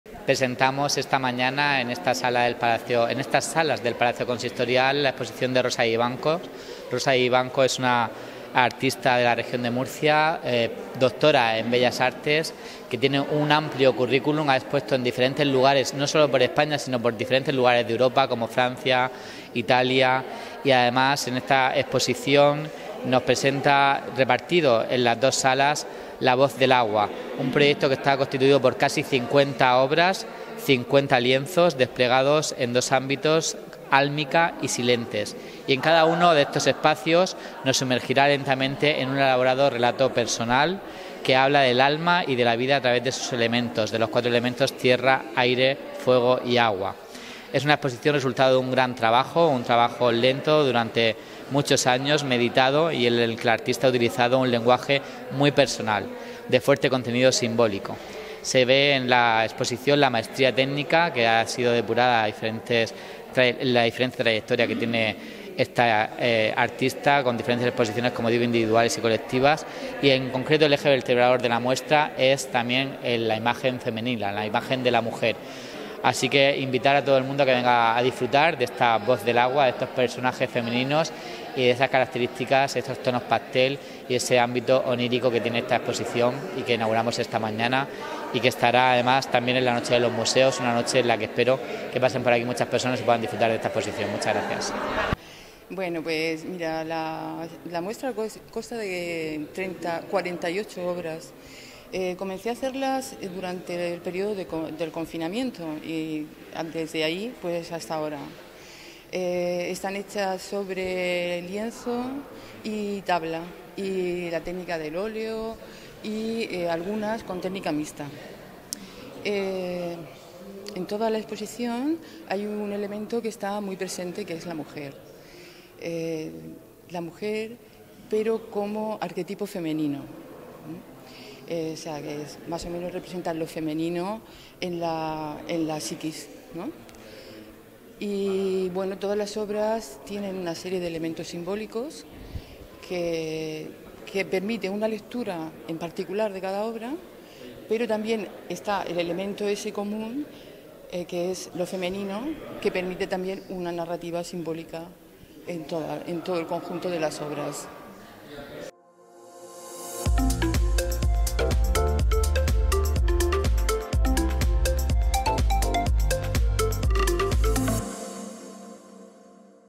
'La voz del agua' de Rosa Vivanco estará expuesta en el Palacio Consistorial hasta el 30 de junio
'La voz del agua', el último trabajo de la artista Rosa Vivanco, se ha inaugurado en el Palacio Consistorial, este jueves 11 de mayo. La muestra, que estará disponible hasta el 30 de junio, se encuentra desplegada en dos ámbitos a lo largo de las salas: Álmica y Silentes. Cada uno de los espacios en los que se divide la muestra, sumerge lentamente al espectador en un elaborado relato personal que habla del alma y de la vida a través de sus elementos: tierra, aire, fuego, agua.

Han inaugurado la exposición el concejal de Cultura, David Martínez Noguera, junto a la artista de la obra, Rosa Vivanco.

"La muestra consta de 48 obras. Empecé a hacerlas durante el periodo de confinamiento, hasta ahora", ha indicado Vivanco. 48 lienzos en los que la artista ha utilizado un lenguaje personal de fuerte contenido simbólico, convirtiendo a la mujer en protagonista indudable, pero aclara, "como arquetipo femenino".

Un concepto universal que aparece representado por mujeres cuyas miradas, en la mayoría de los cuadros, se mantienen perdidas en un espacio indefinido o son dirigidas de soslayo a su alrededor. Son mujeres serenas, elegantes, conmovedoramente silenciosas, que giran levemente su cabeza sobre el hombro, como si acabaran de descubrir la presencia del espectador que las contempla inmersas en un mundo mágico.

Su maestría técnica, depurada a lo largo de numerosas exposiciones individuales y colectivas, nacionales e internacionales, le ha permitido transferir directamente a los lienzos un mundo profundo y rico en significados, apelando a la subjetividad del espectador que después de una primera mirada, comprobará que se encuentra ante un sutil enigma.

La obra de Rosa Vivanco describe espacios imaginarios que transmiten una quietud atemporal. Su dominio de la expresión plástica, del dibujo, el color y la textura, le permite crear atmósferas en las que quedan envueltos seres y objetos extraordinarios. Adentrarse en Álmica permite contemplar un universo de imágenes y poesía, casas en llamas, semillas de luna, faros del aire, peces con rostro, árboles de agua, esferas estelares, miradas que se mueven como peces.